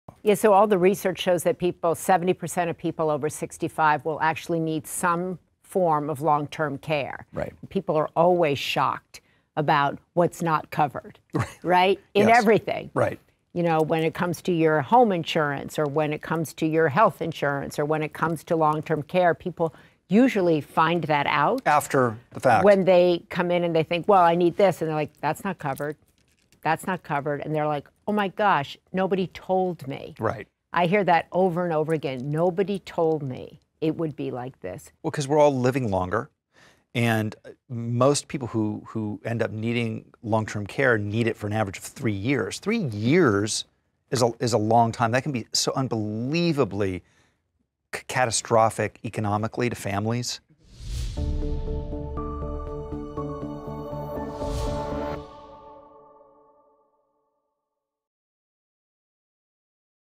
Maria Shriver: The Shock of Long Term Care | seniorcarehelper.com
Maria Shriver and Rob Lowe discuss how long term care can become a real shock in peoples lives.